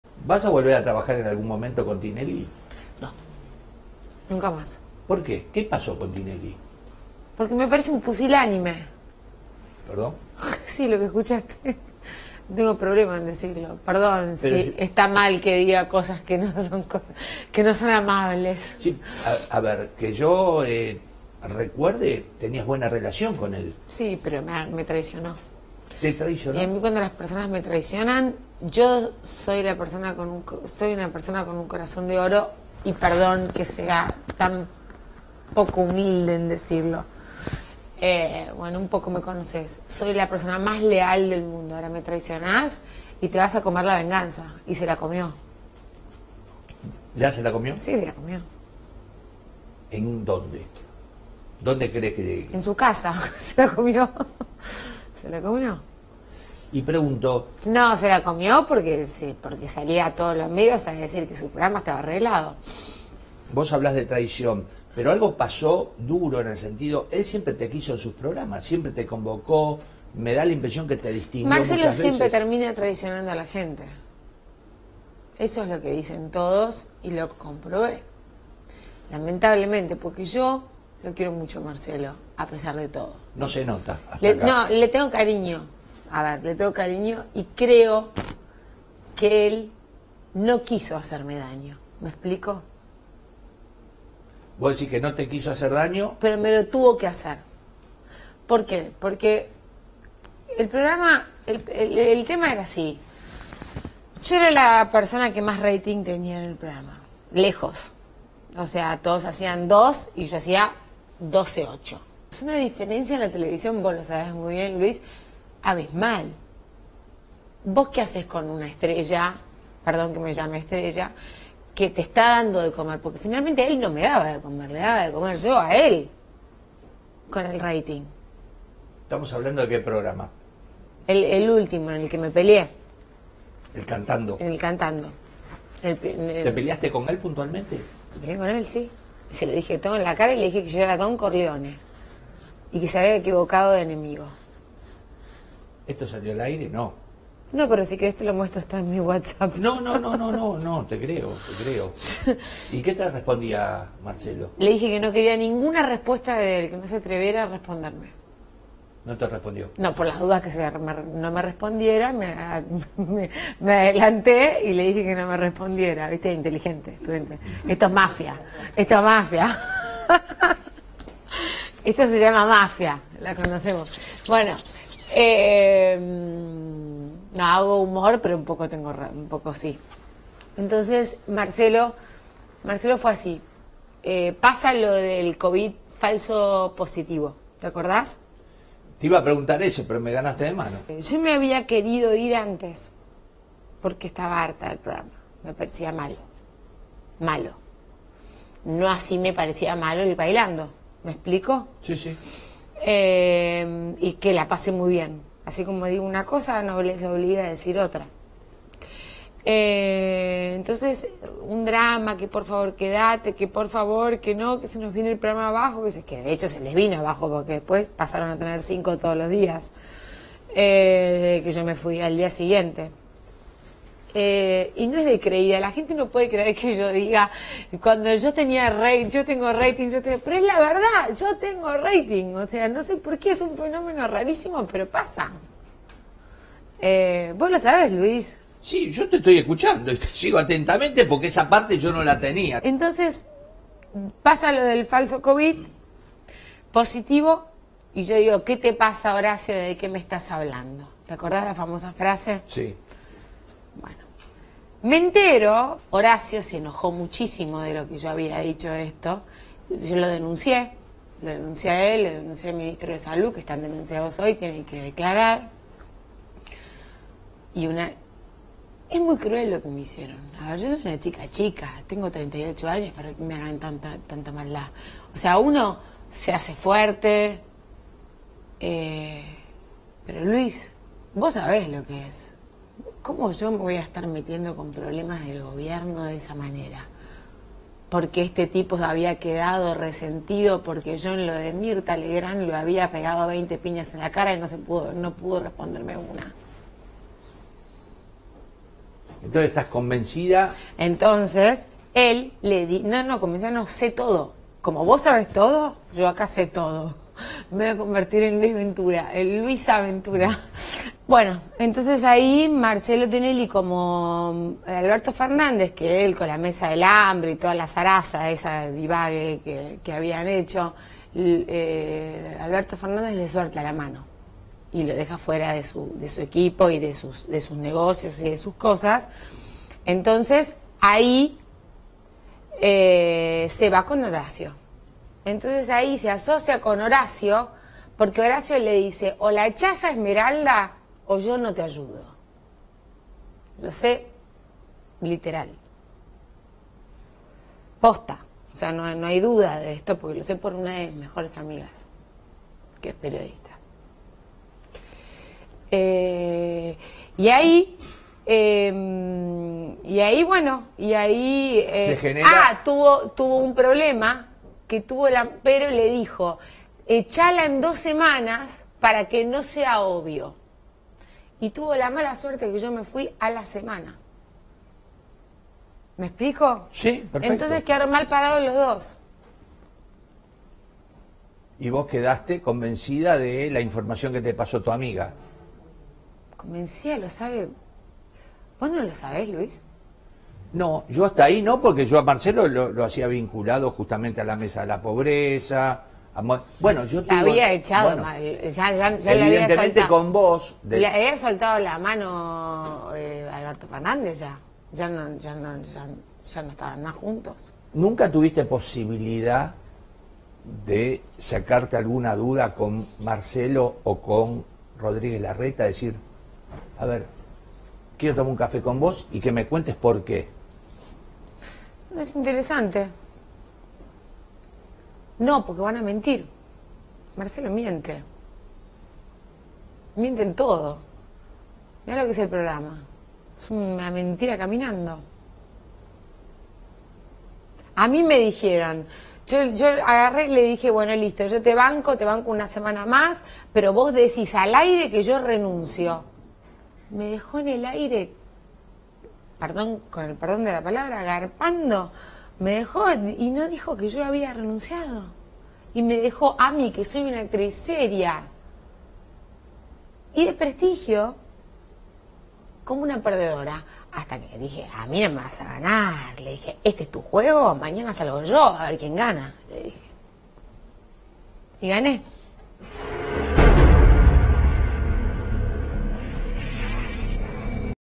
Esmeralda Mitre: "No volvería a trabajar con Tinelli"
La actriz atacó contra Marcelo Tinelli